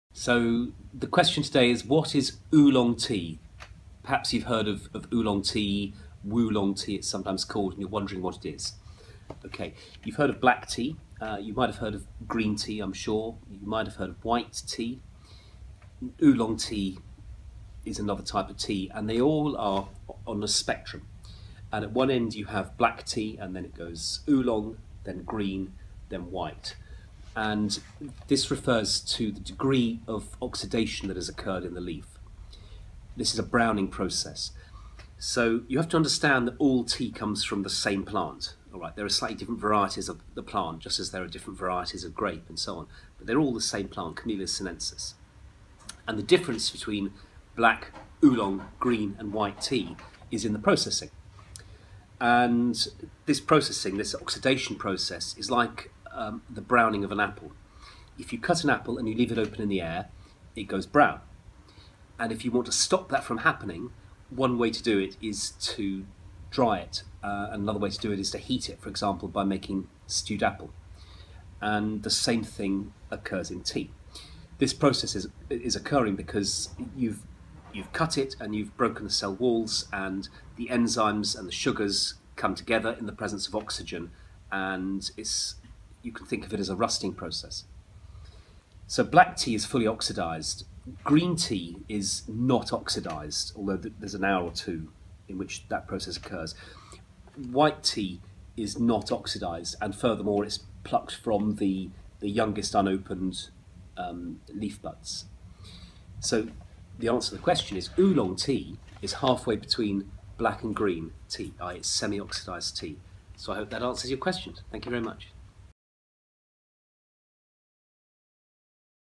What is Oolong Tea / Wu-long Tea ? (Understanding Tea Types) - Tea Q&A with Andrew Gadsden
** Write your question in the comments below **